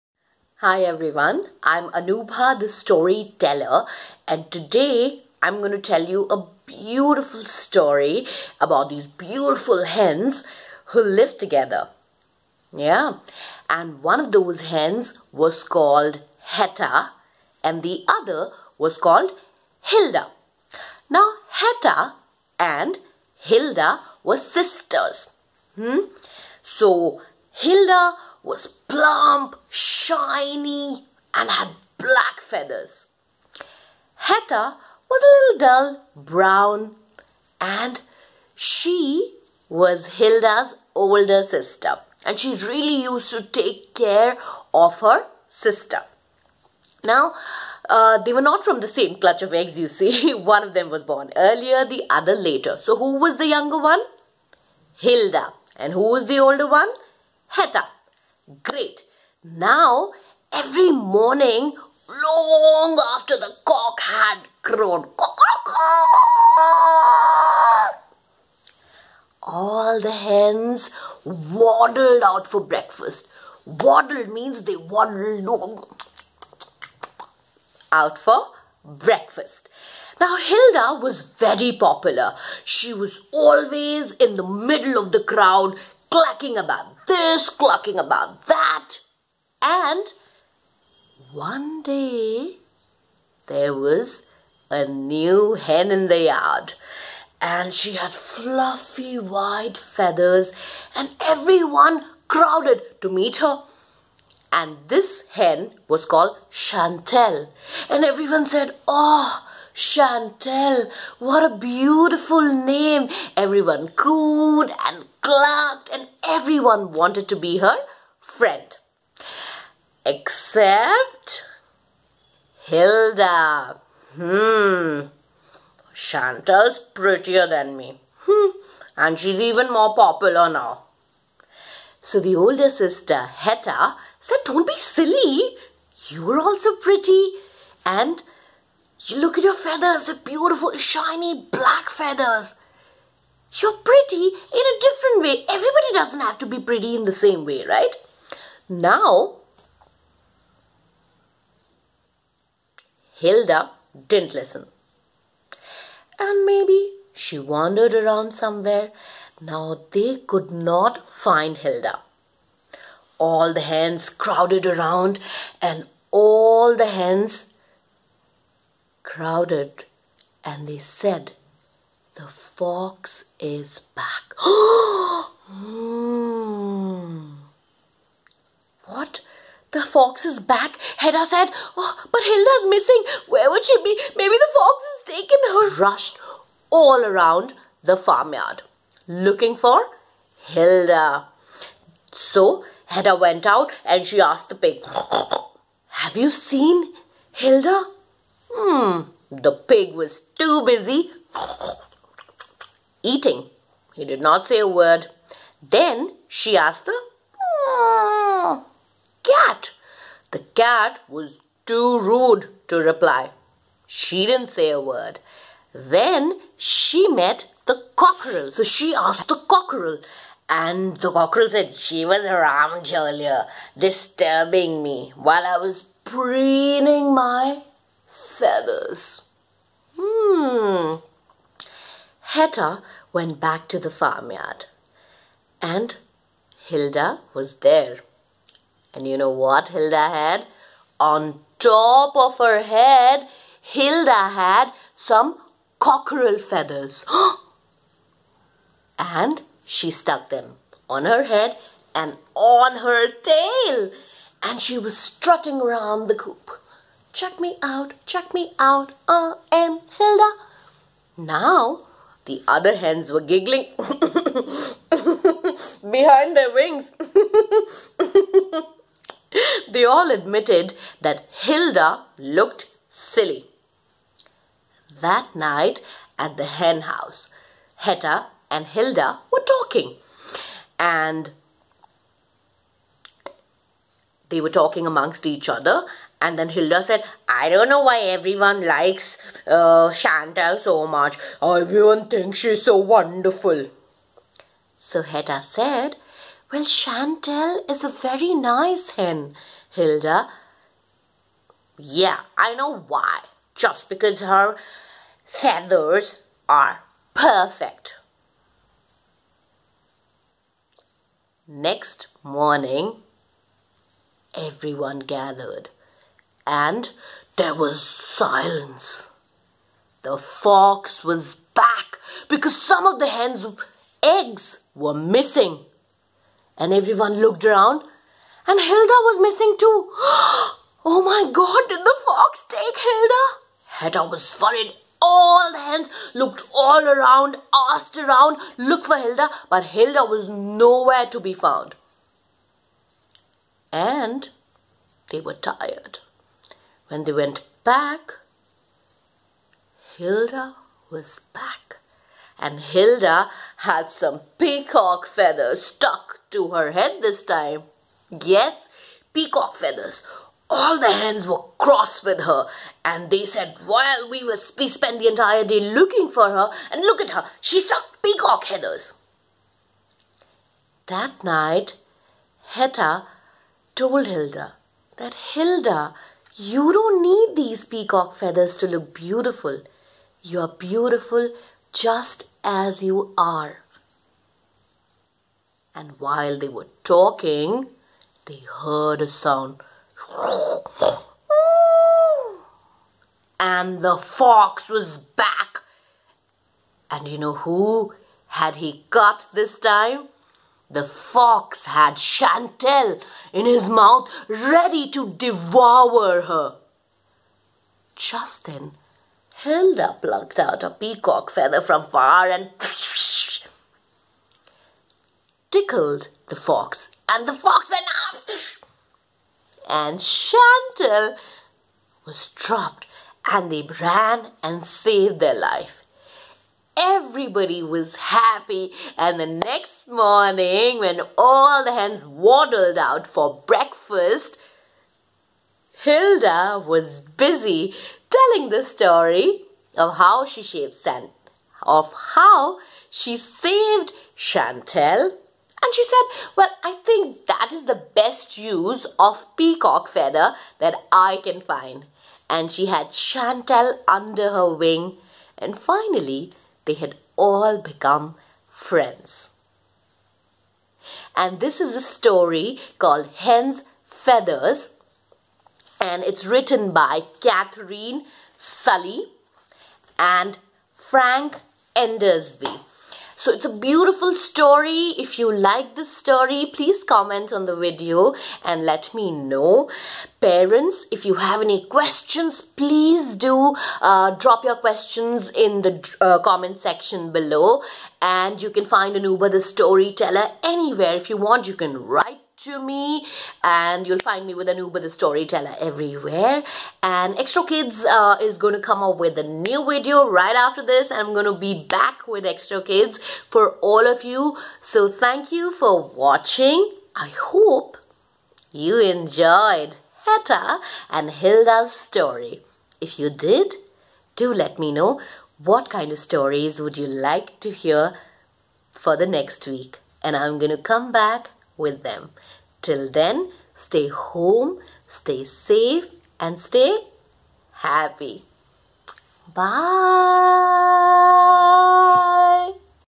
HEN'S FEATHERS II SHORT STORY II ANUBHA THE STORYTELLER
Why are we threatened by others? What do we do, when someone better comes along? A children's story that asks difficult questions, do listen and share your views.

CREDITS :
Published by : Alligator Products Ltd
Written by : Katherine Sully
Illustrated by : Frank Endersby

Do  not use this video/part of the video without permission.

Love - Anu